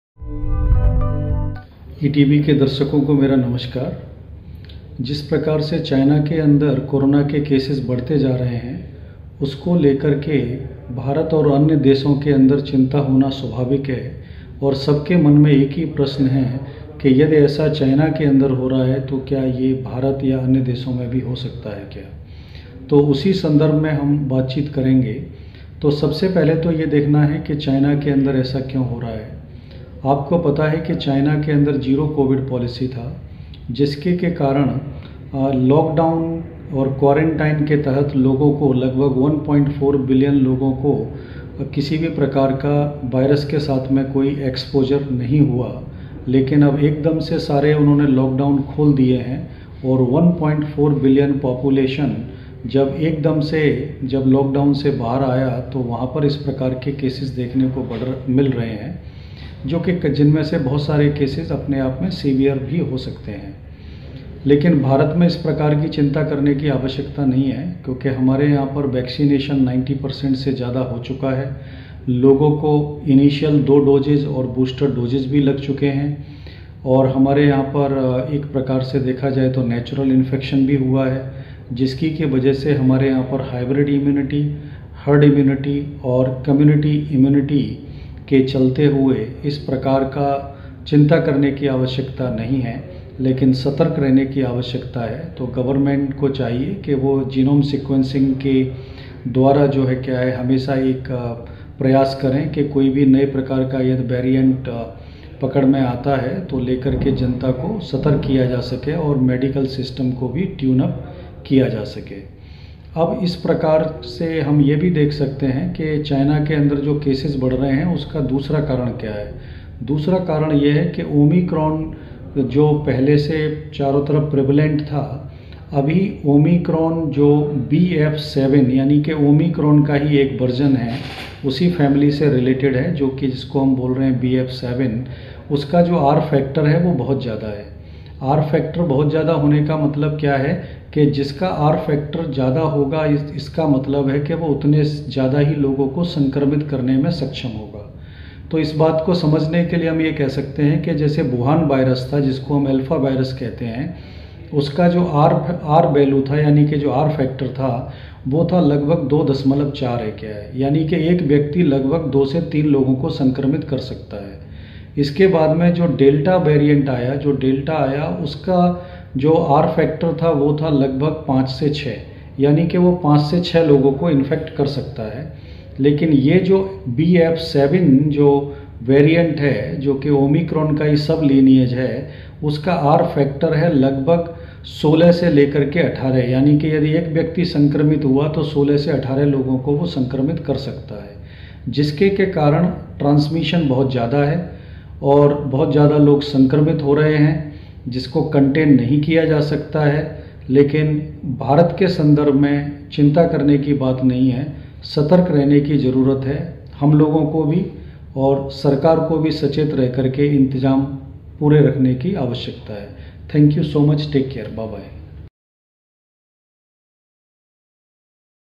Concerns of Delhiites increase due to Corona explosion in China, experts say no need to panic
The cases of Covid-19 are under control in Delhi, but the rate at which corona cases have been increasing in China, is generating fear among people in the capital.